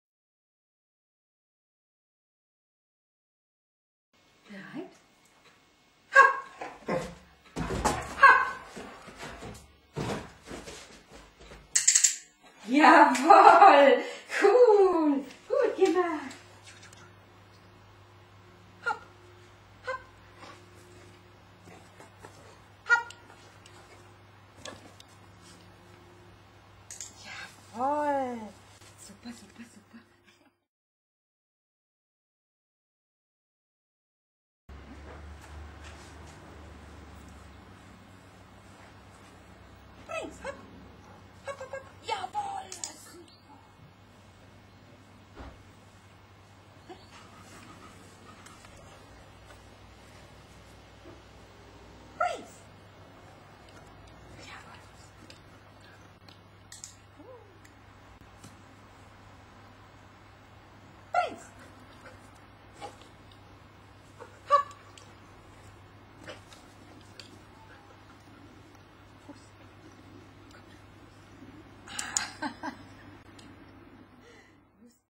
Obedience - die Hürde
Sprung über die Hürde und Apportieren über die Hürde